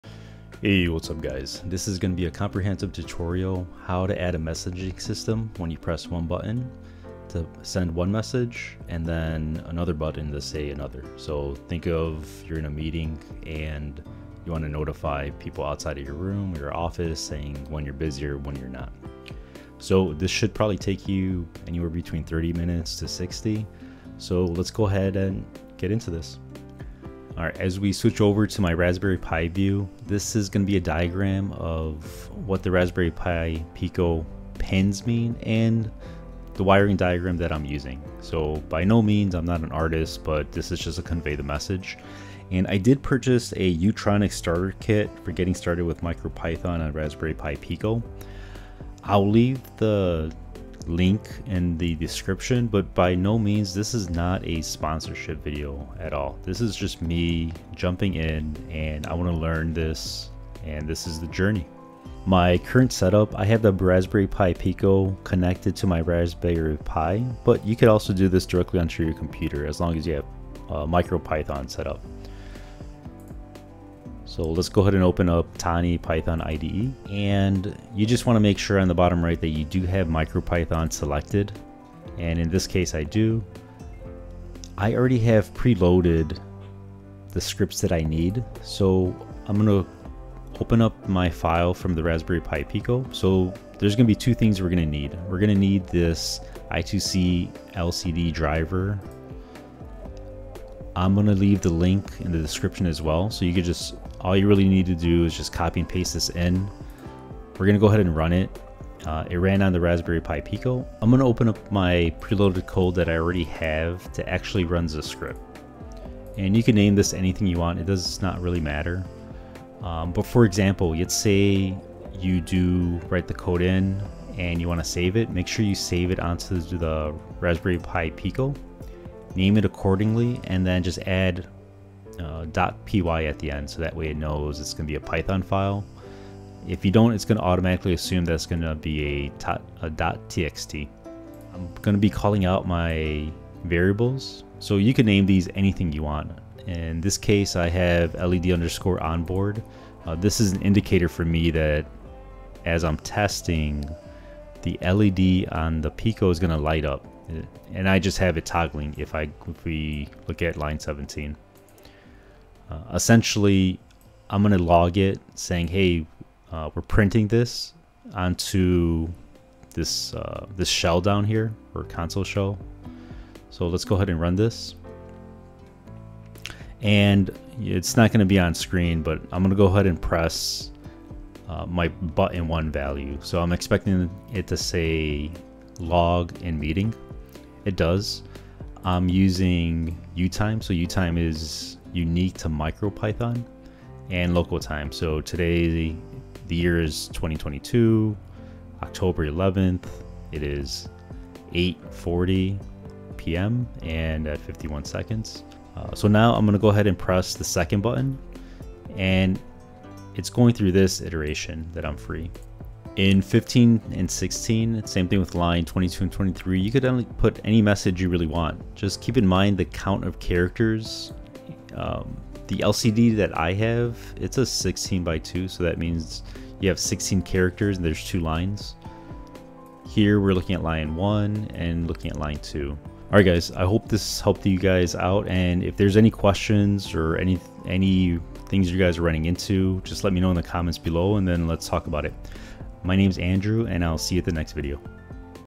Raspberry Pi Pico LCD Messenger with Buttons Tutorial
Create a messaging system through the Raspberry Pi Pico with buttons and an LCD Screen.

Hardware Needed:
Raspberry Pi Pico
2x Buttons
Jumper cables
Power Source
LCD

Kit I used for this project is called "UCTRONICS Starter Kit for Getting Started with Micropython on Raspberry Pi Pico"

Video Contents
Intro/Demo: 00:00
Wiring Diagram: 00:26
Programming: 01:17